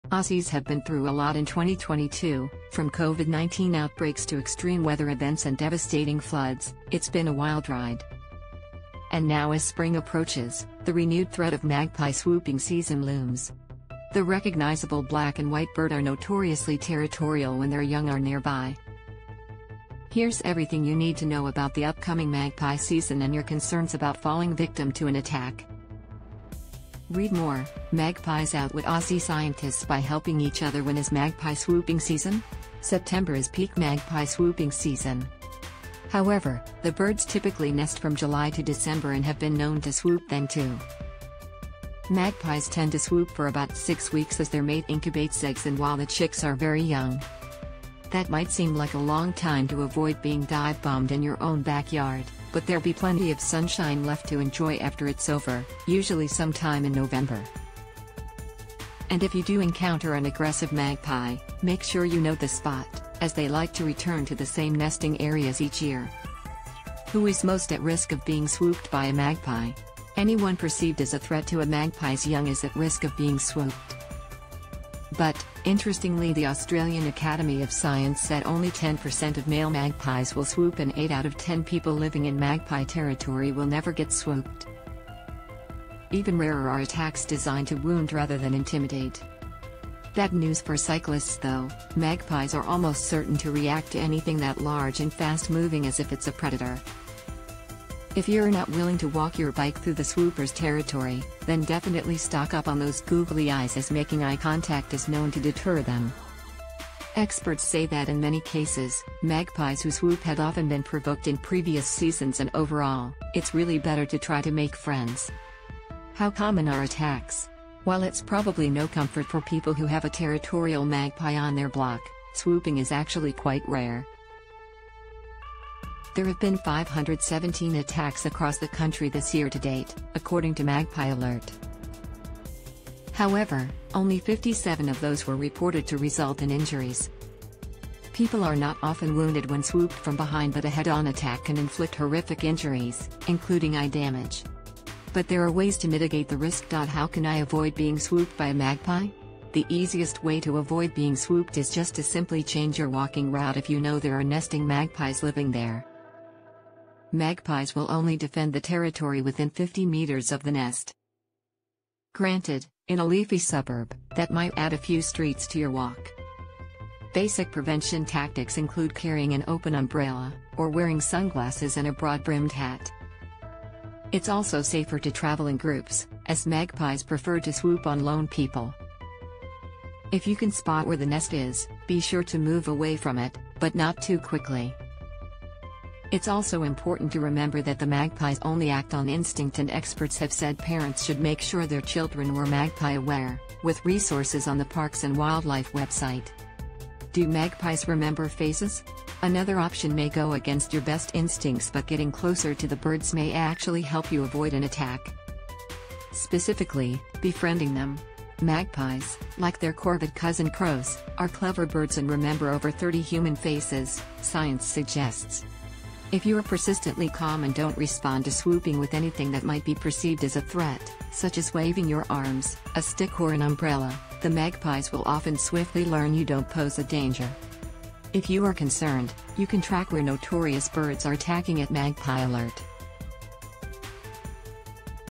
Here's how to avoid magpie attacks this swooping season
Aussies have been through a lot in 2022, from COVID-19 outbreaks to extreme weather events and devastating floods - it's been a wild ride.And now as spring approaches, the renewed threat of magpie swooping season looms.The recognisable black and white bird are notoriously territorial when their young are nearby.Here's everything you need to know about the upcoming magpie season and your concerns about falling victim to an attack.READ MORE: Magpies outwit Aussie scientists by helping each otherWh